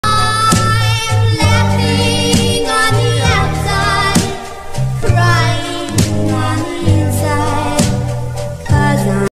Garota de pedra - Stone Girl
Hello pessoal, hoje vocês vão conferir eu reproduzindo uma make de pedra rachada!!